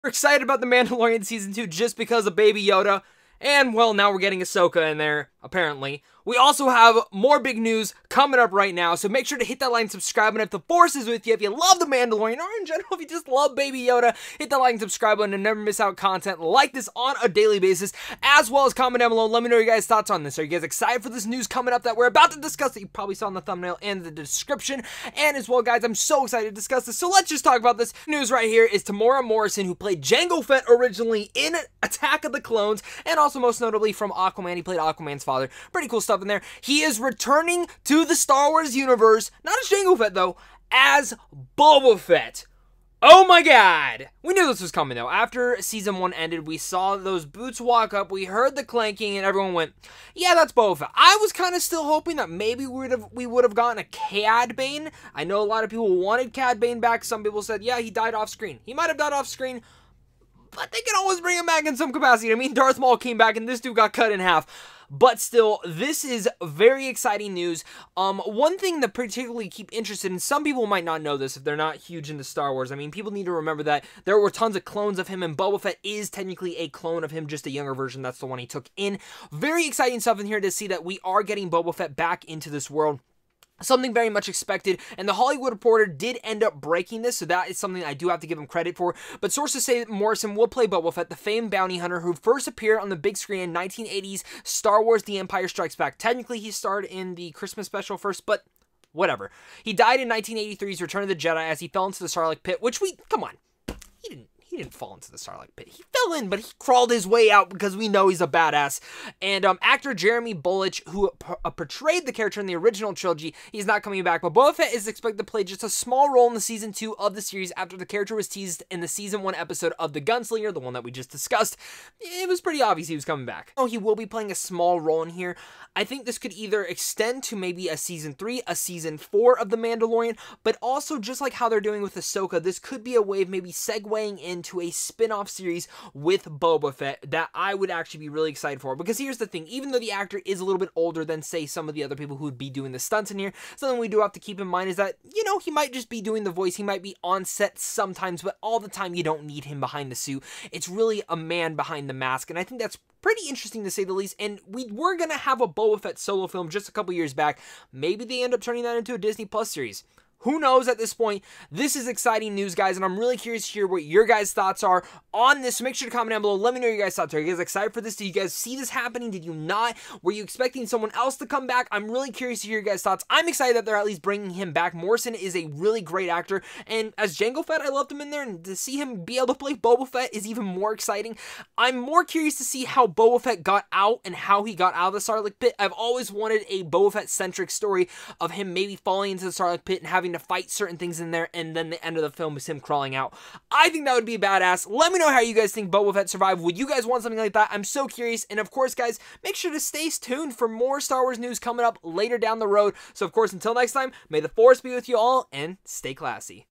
Boba Fett CONFIRMED for Mandalorian Season 2
HOLY HELL we knew it was coming but now we have direct news that BOBA FETT will be in MANDALORIAN SEASON 2. Thanks to the Hollywood Reporter for breaking this news! Temuera Morrison will play BOBA FETT in Mando Season 2.